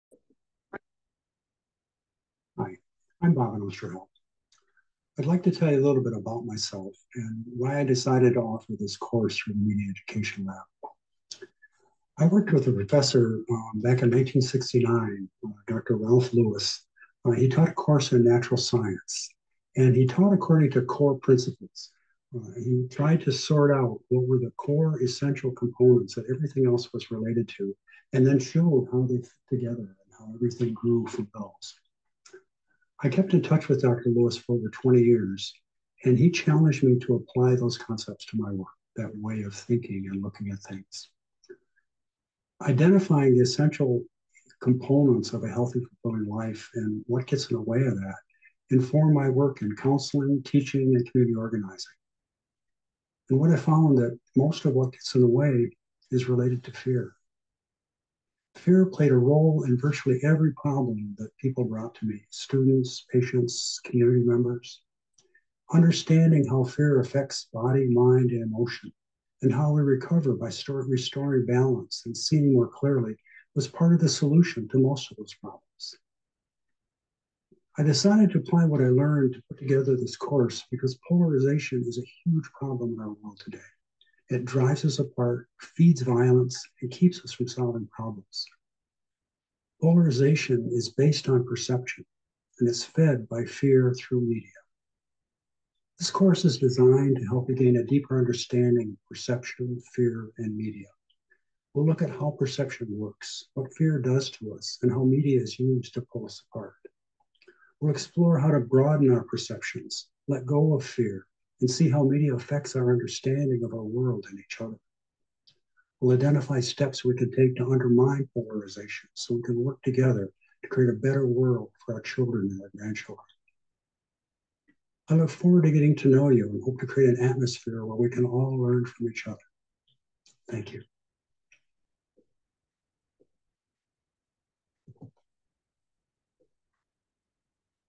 Perception, Fear, and Media: Roots of Polarization -- Introducing your instructor
Welcome video for an online course offered through the Media Education Lab at the University of Rhode Island July 21-Aug 2, 2024.